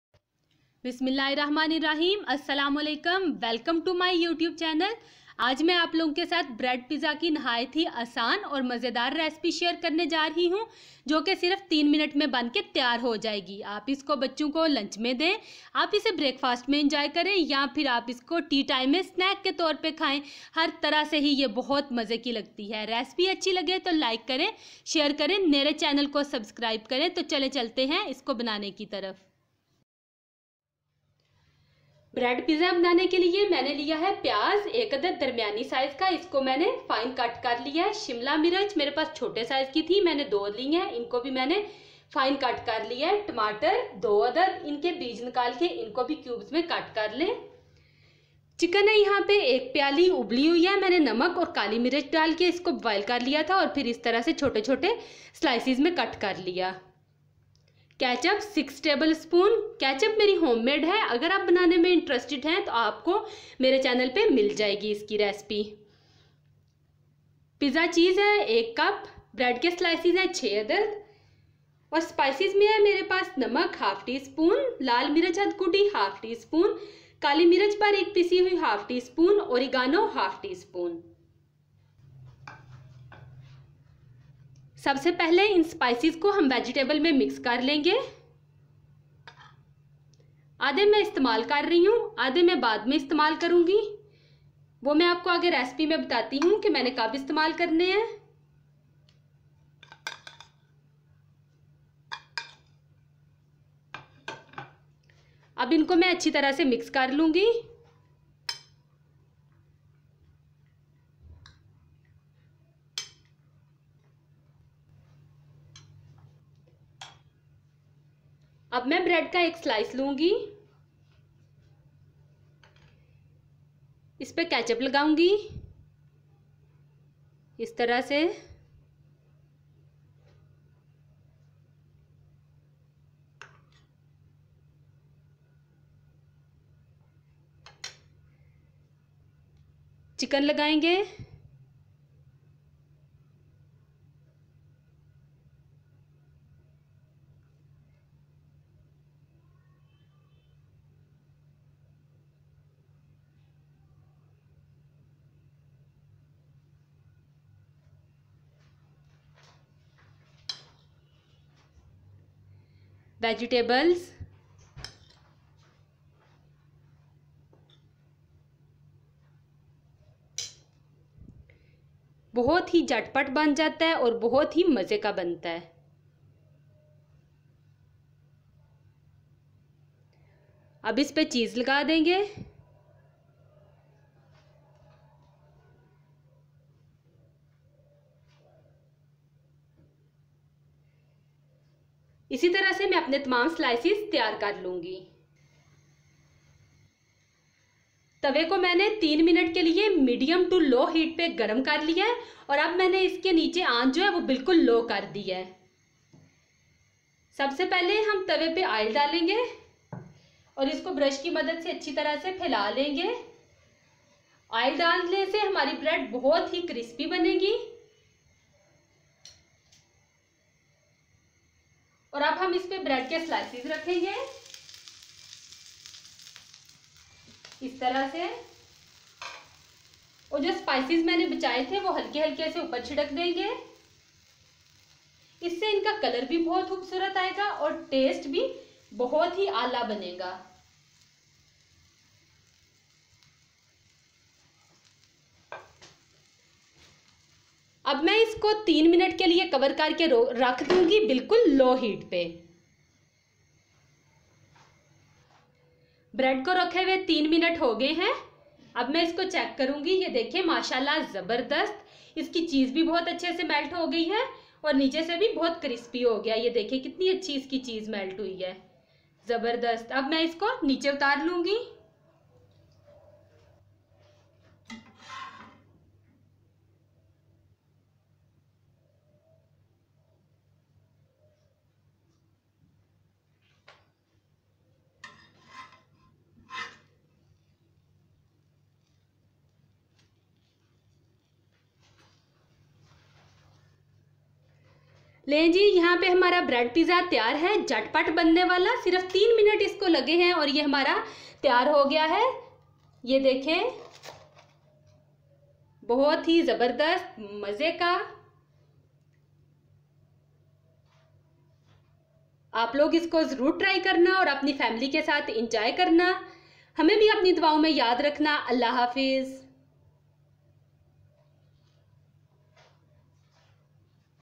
Bread Pizza on Tawa Recipe 🍞🍕 | Bread Pizza Banane ka Tarika
It is very easy and quick way to make pizza at home without oven.